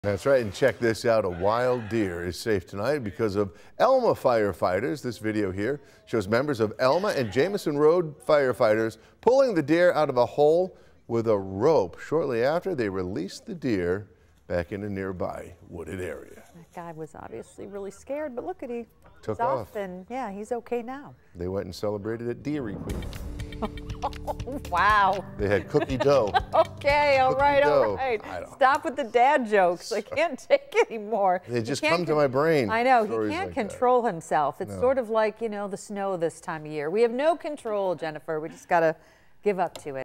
WNY firefighters rescue deer from well
Fire company members helped the furry friend to safety.